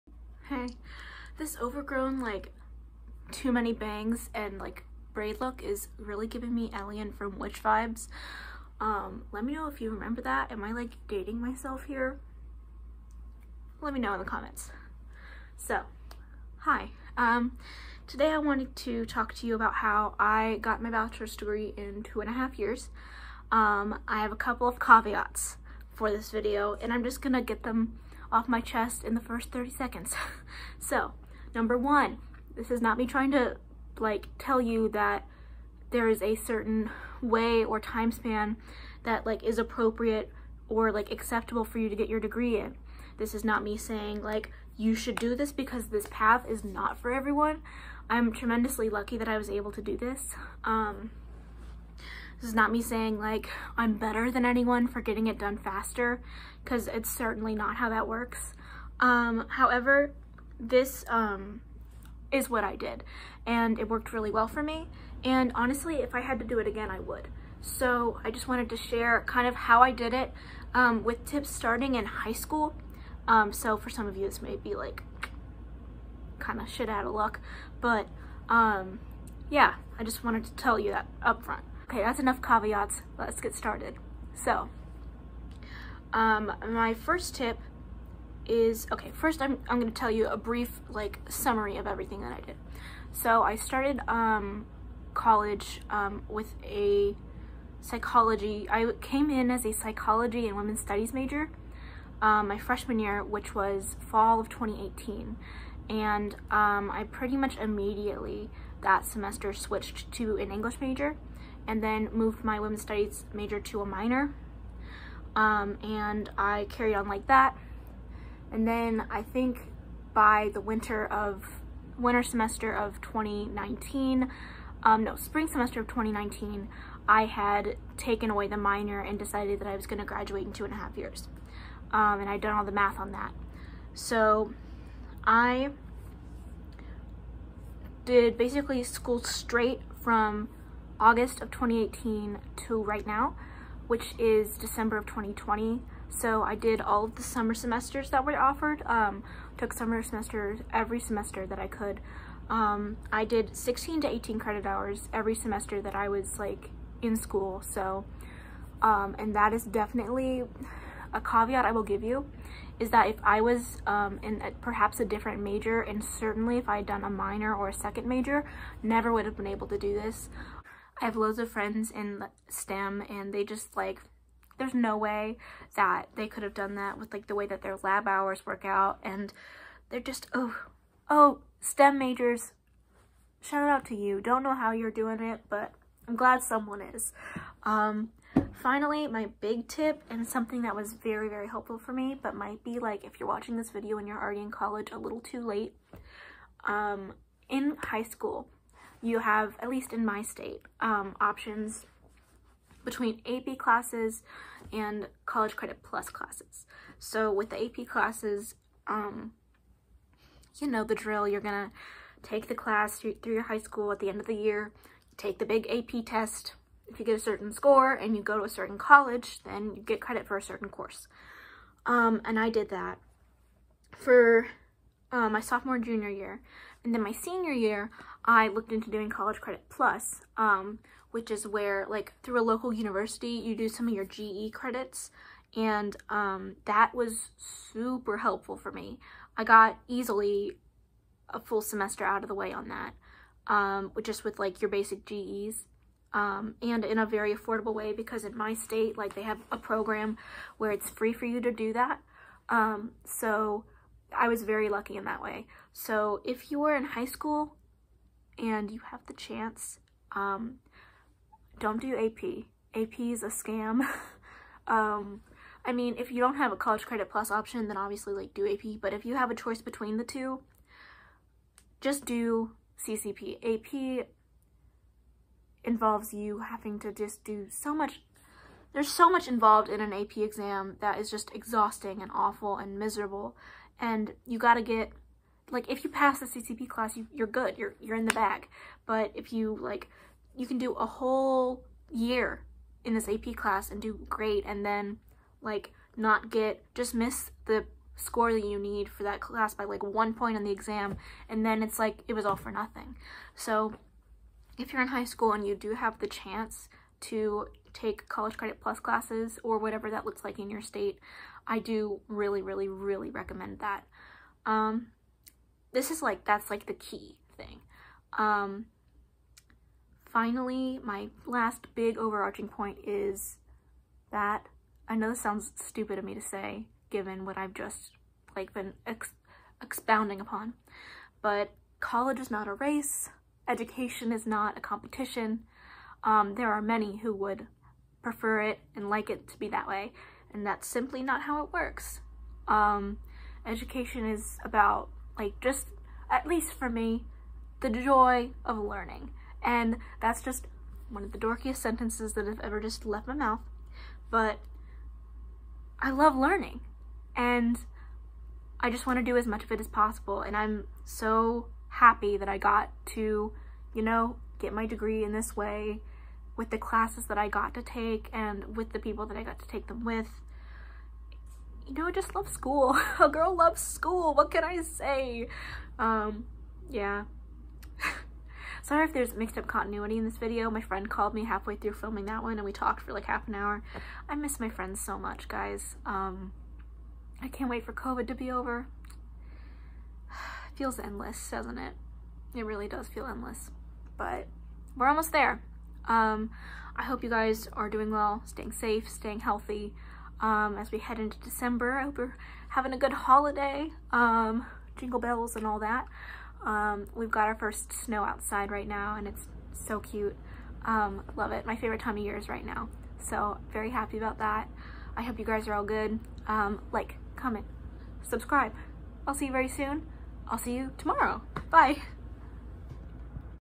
how i got my bachelor’s in 2.5 years (cc)
here’s how i finished my bachelor’s degree in two and a half years!!
hope it helps someone